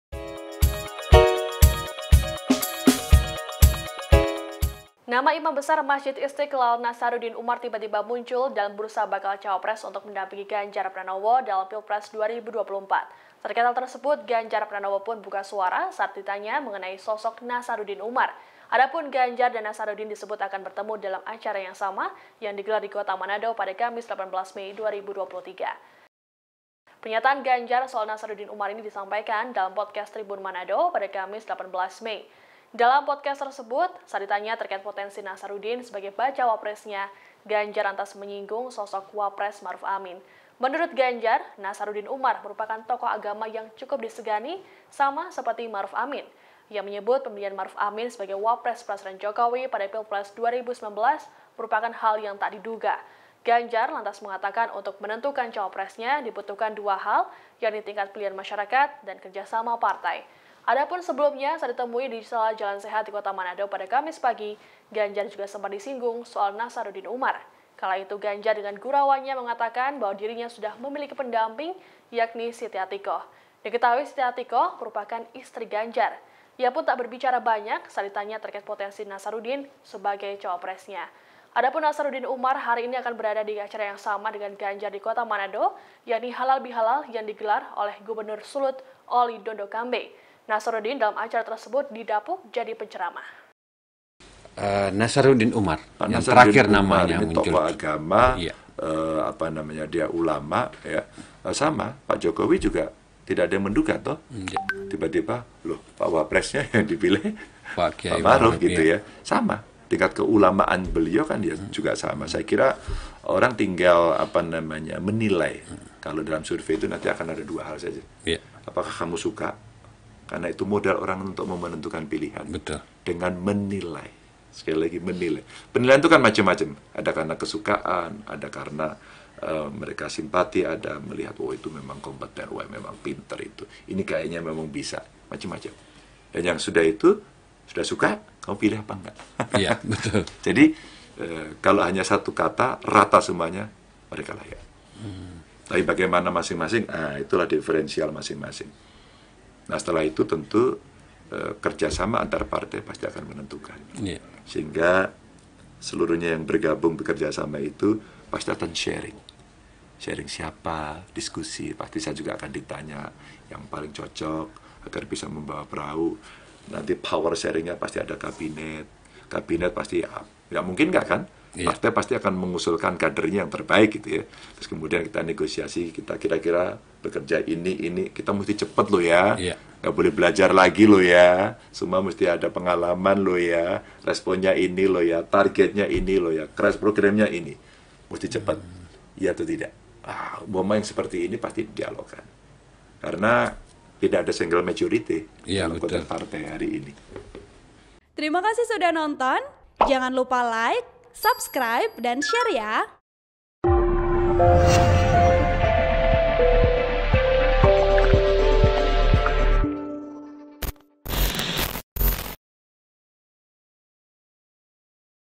Ditanya soal Nasaruddin Umar, Ganjar Pranowo Singgung Ma'ruf Amin: Tingkat Keulamaannya Sama
TRIBUN-VIDEO.COM - Nama Imam Besar Masjid Istiqlal Nasaruddin Umar tiba-tiba muncul dalam bursa bakal cawapres untuk mendampingi Ganjar Pranowo dalam Pilpres 2024.

Terkait hal tersebut, Ganjar Pranowo pun buka suara saat ditanya mengenai sosok Nasaruddin Umar.

Adapun Ganjar dan Nasaruddin disebut akan bertemu dalam acara yang digelar di Manado pada Kamis (18/5/2023).

(TribunVideo.com)